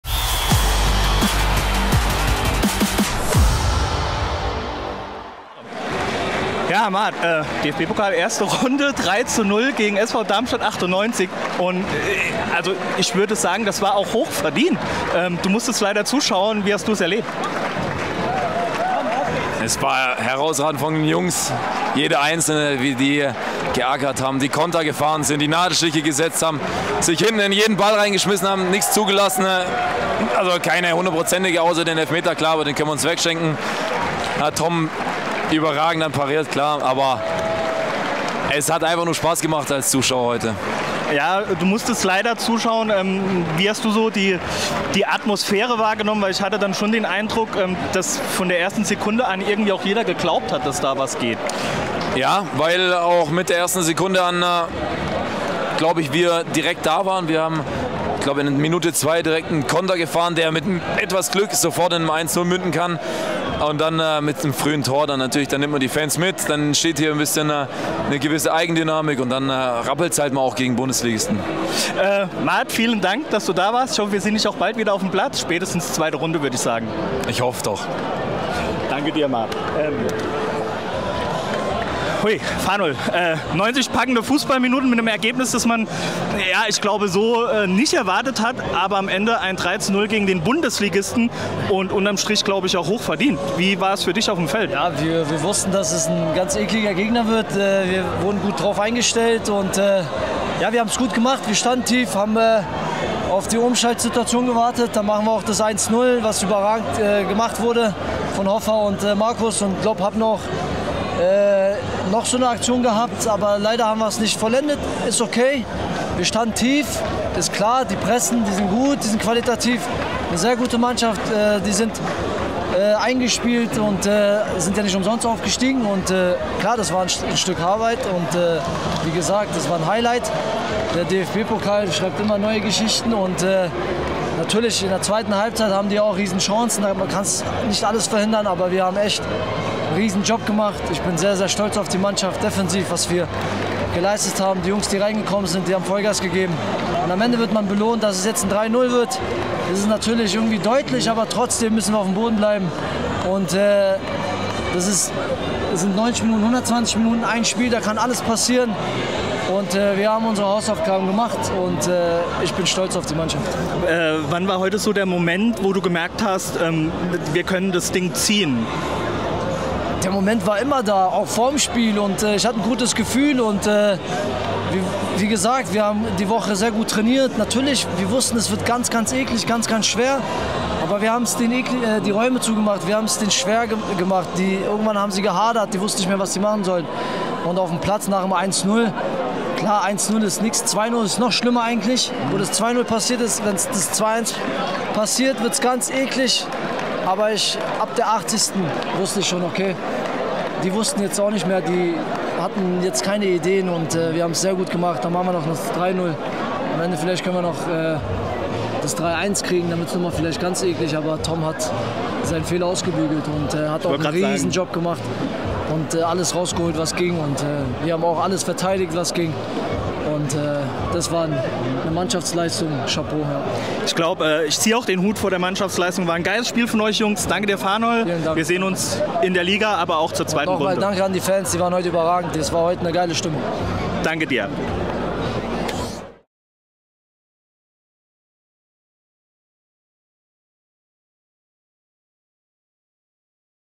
Homburg - Darmstadt: Stimmen zum Spiel I
Stimmen zum Spiel: FC 08 Homburg - SV Darmstadt 98
Saison 2023/24: DFB-Pokal
Interviewpartner: Mart Ristl und Fanol Perdedaj